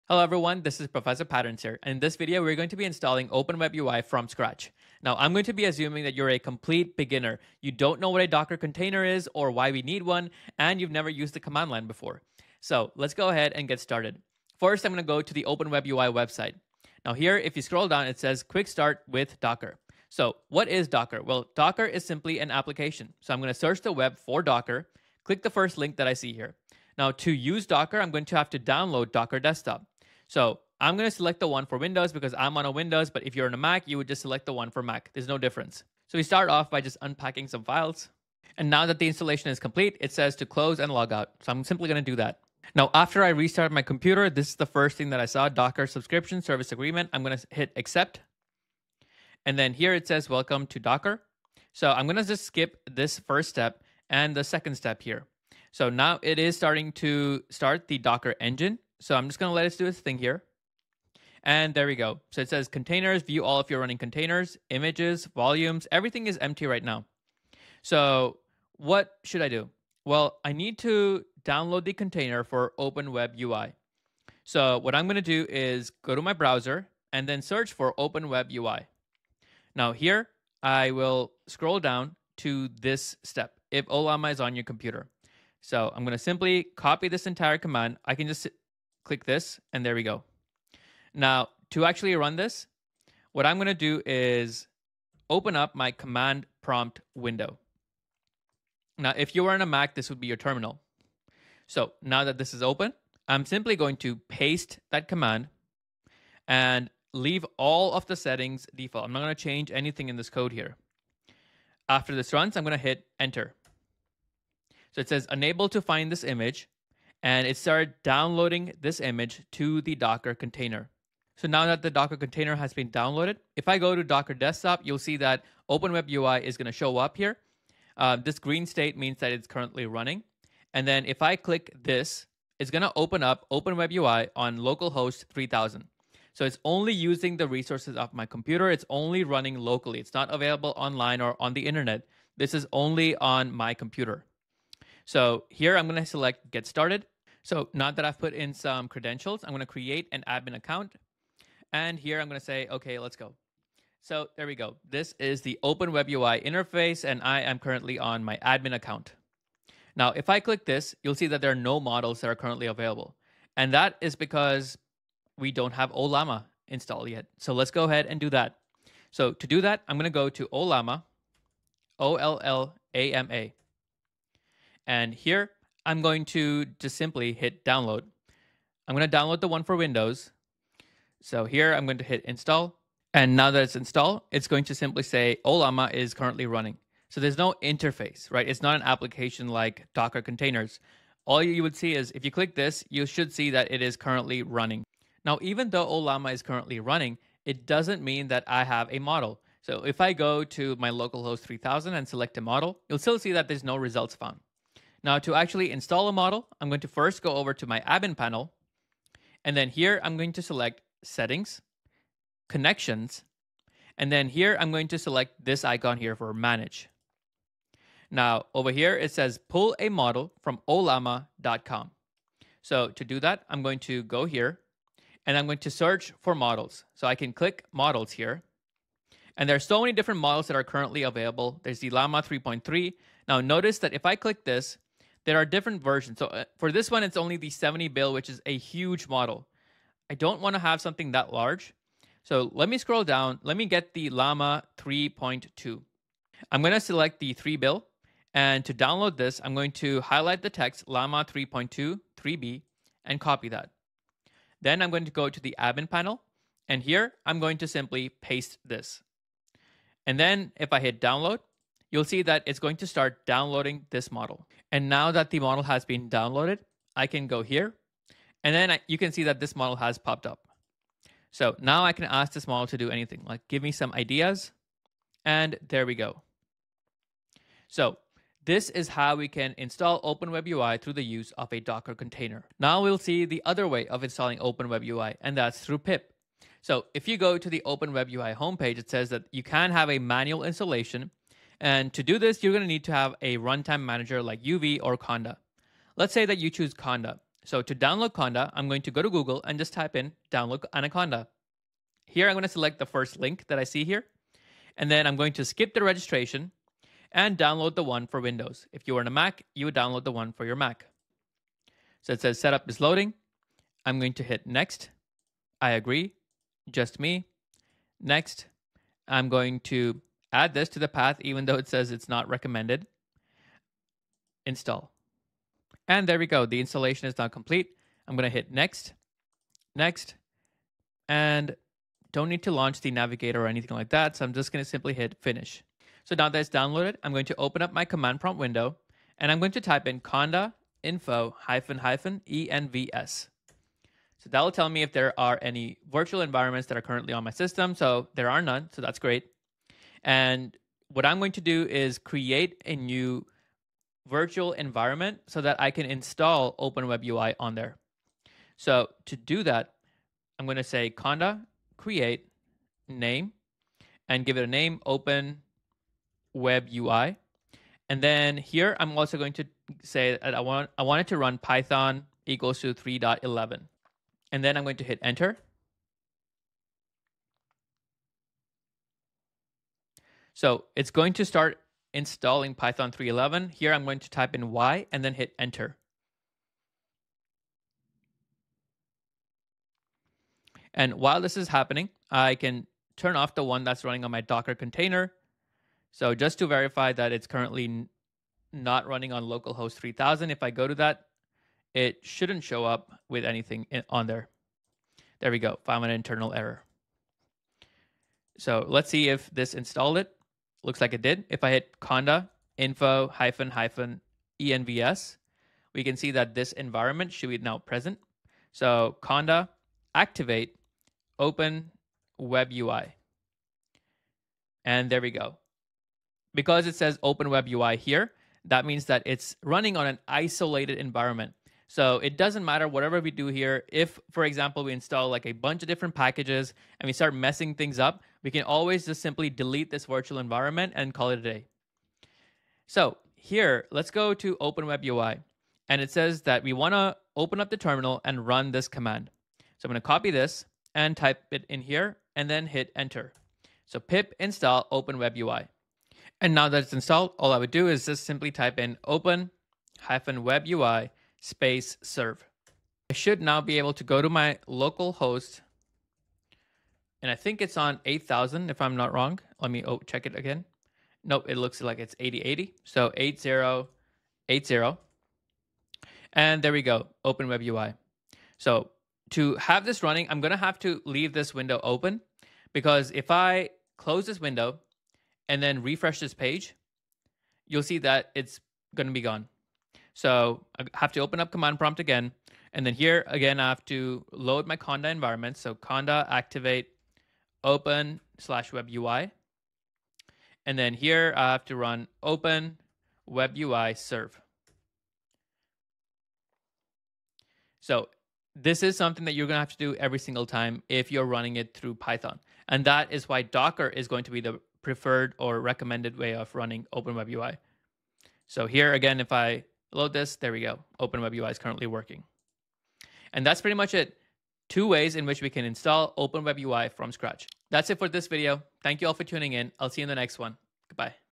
Beginners Guide to Installing Open WebUI
🔧 Learn how to install and set up Open Web UI from absolute scratch, perfect for complete beginners! This comprehensive tutorial walks you through two different installation methods, ensuring you can choose the approach that works best for your needs.
📚 In this detailed guide, we cover:
TIME STAMPS:
00:00 - Introduction
00:14 - Docker Installation Method
02:39 - Installing Ollama
03:24 - Adding Your First Model
04:46 - Conda Installation Method
07:36 - Running Open Web UI through Python
08:32 - Comparing Both Methods
DETAILED OVERVIEW:
This tutorial assumes no prior knowledge and guides you through every step of the installation process. We begin with the Docker method, explaining what Docker is and why it's beneficial for running Open Web UI. Learn how to install Docker Desktop, pull the necessary containers, and get your first AI model up and running.
The second half of the tutorial covers the manual installation method using Conda, perfect for those who prefer working directly with Python packages. We'll show you how to set up a clean virtual environment, install dependencies, and get Open Web UI running locally.
KEY LEARNING POINTS:

Understanding Docker containers and their benefits
Setting up Docker Desktop from scratch
Installing and configuring Ollama
Downloading and implementing AI models
Creating and managing Conda environments
Installing Python packages safely
Running Open Web UI on different ports
Troubleshooting common installation issues

REQUIREMENTS:

Windows or Mac computer
Internet connection
No prior experience needed!

💡 PRO TIP: The Docker installation method is recommended for beginners as it requires less maintenance and provides a more consistent environment.
Have questions? Drop them in the comments below, and we'll help you troubleshoot any installation issues you encounter.